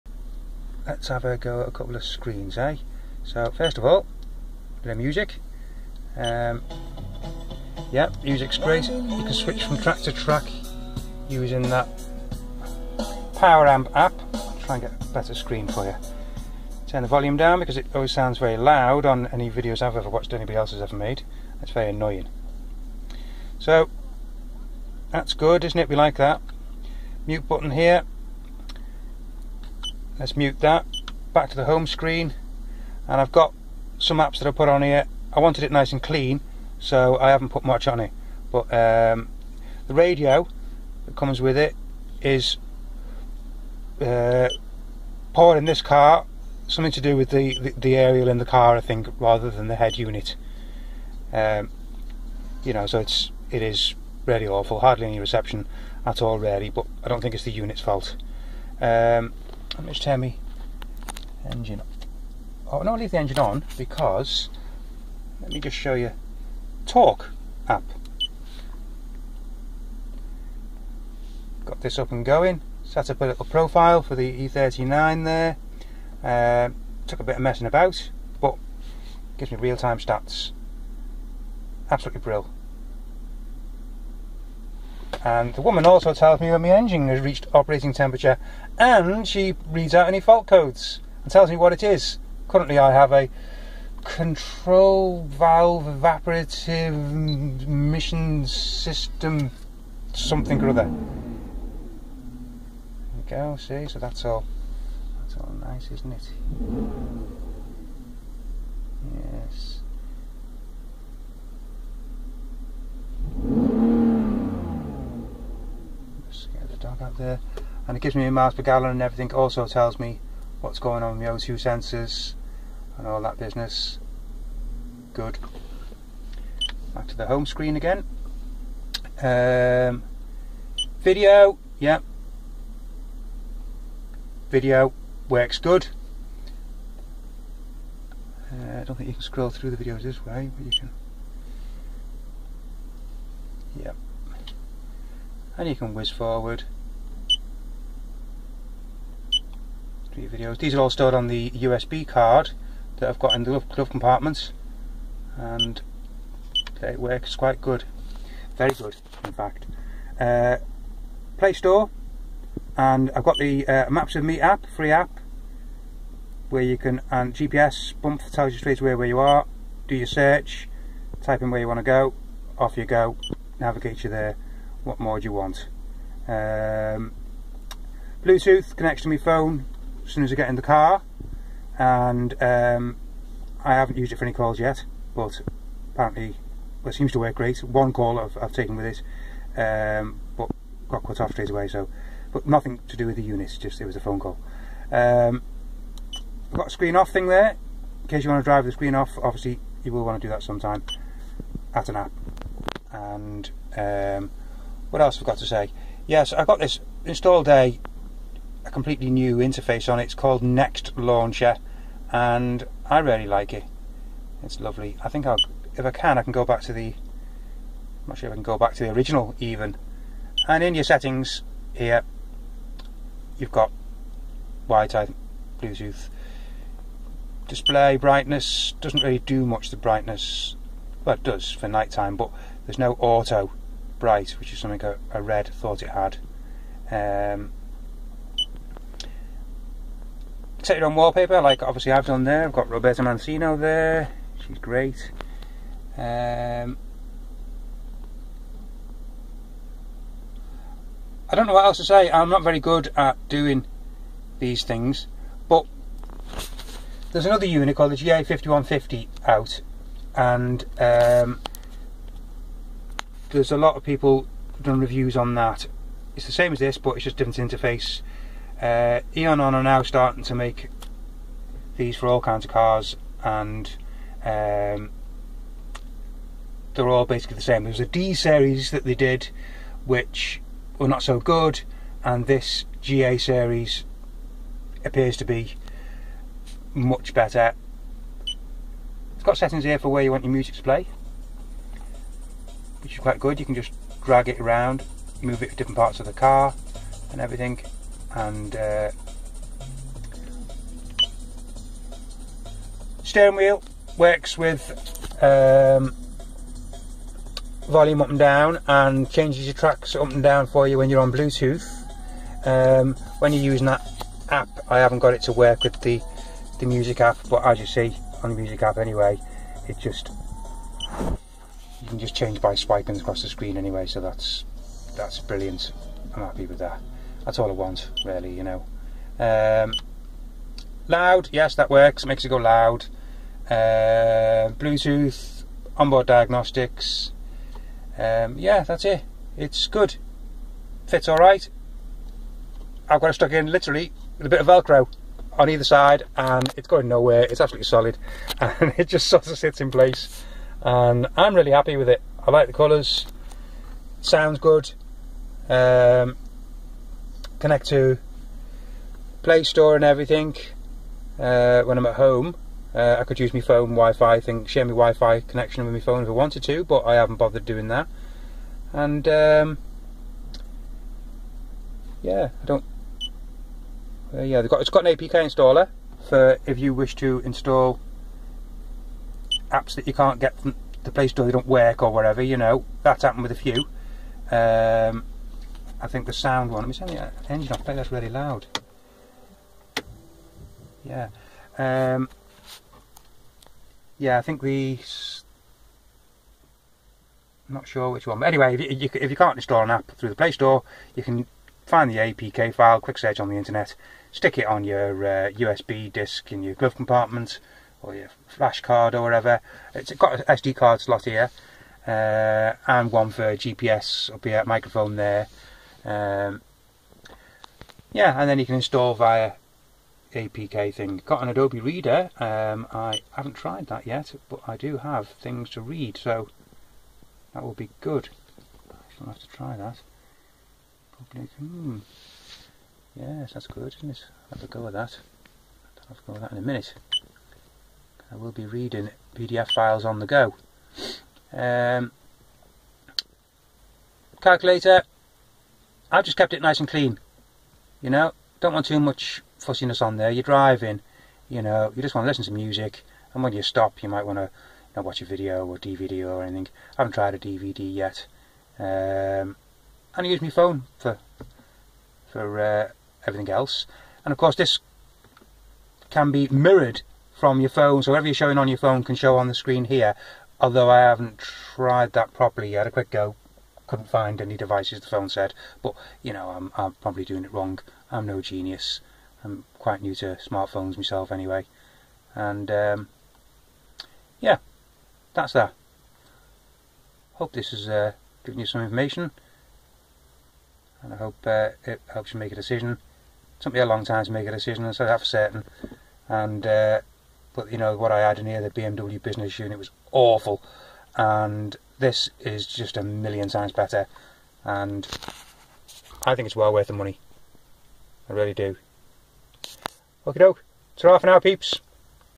Eonon GA5166 Android in BMW 5 Series - fits E39/E53
Replacement touch screen android 4.2.2. head unit that fits BMW 5 Series and X5.  I'm using "Next Launcher" and "Poweramp" for the interface.  Plays DVD's, can connect to play store, WIFI, GPS, Will format HD video for the screen, bluetooth, steering control compatible (mostly), USB/iPod in, RCA outs and subwoofer out. comes with cables for round pin and flat pin.  Problems fitting if you have DSP, factory NAV, factory amp. (accessory cable to the back of the car needed)